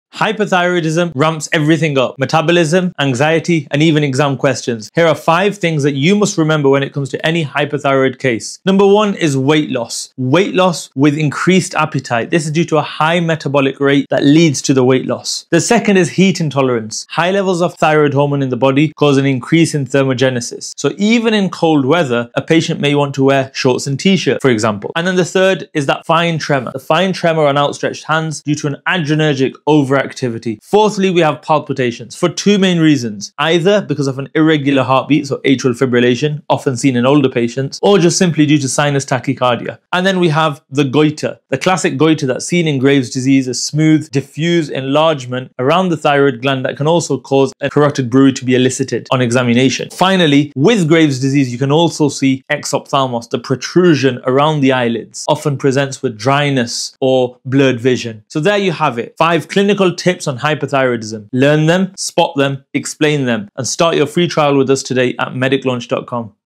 How to Spot Hyperthyroidism Fast: 5 High-Energy Signs You Can’t Ignore 🔥
🎬 5 Signs of Hyperthyroidism You Can’t Miss 🔥

Weight loss, heat intolerance, palpitations? These classic clues point to an overactive thyroid.

Don’t overlook the subtle signs.

💡 Spot it early. Manage it right.